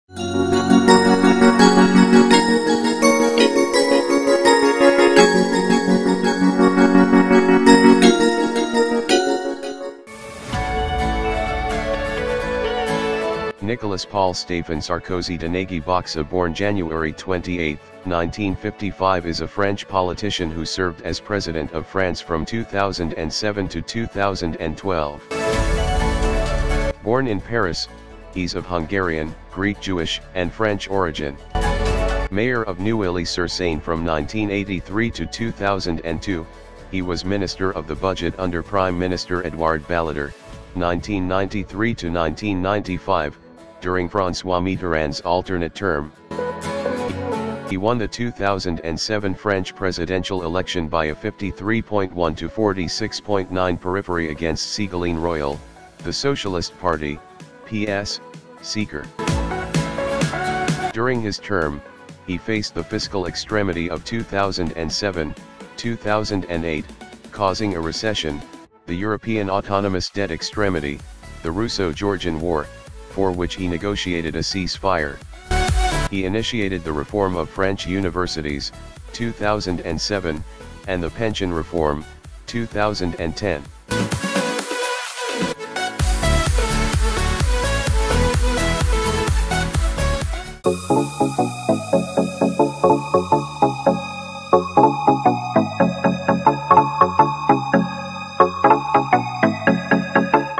Nicolas Sarkozy
Nicolas Paul Stéphane Sarközy de Nagy- Bocsa born 28 January 1955 is a French politician who served as President of France from 2007 to 2012. Born in Paris, he's of Hungarian, Greek Jewish, and French origin. Mayor of Neuilly- sur- Seine from 1983 to 2002, he was Minister of the Budget under Prime Minister Édouard Balladur( 1993 – 1995) during François Mitterrand's alternate term.

He won the 2007 French presidential election by a53.1 to46.9 periphery against Ségolène Royal, the Socialist Party( PS) seeker. During his term, he faced the fiscal extremity of 2007 – 2008( causing a recession, the European autonomous debt extremity), the Russo- Georgian War( for which he negotiated a ceasefire). He initiated the reform of French universities( 2007) and the pension reform( 2010).